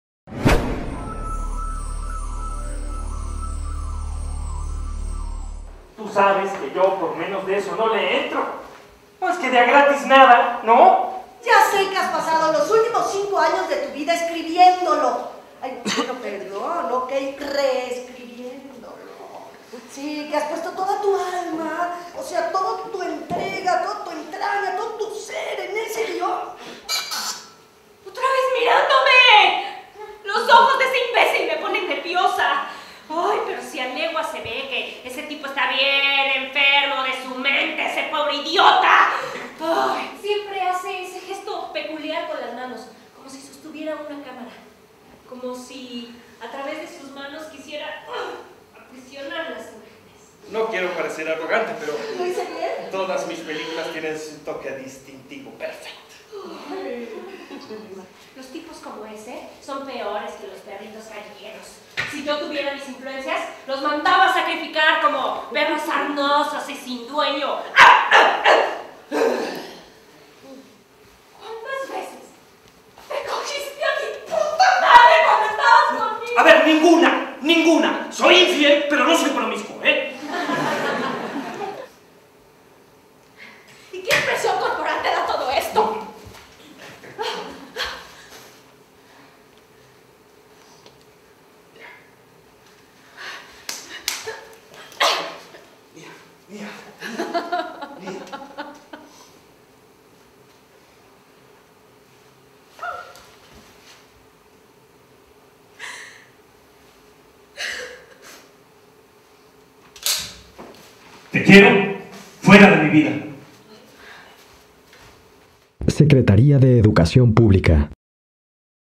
Connecting People
La Coordinación Nacional de Teatro del Instituto Nacional de Bellas Artes anuncia la nueva temporada de la obra Connecting People de la dramaturga mexicana Elena Guiochins, bajo la dirección de Kikka-Lisa Livanainen.
Se presenta en el Teatro El Granero, Xavier Rojas, hasta el 28 de julio en el Centro Cultural del Bosque, los jueves y viernes a las 20:00 horas, sábado a las 19:00 horas y domingo a las 18:00 horas.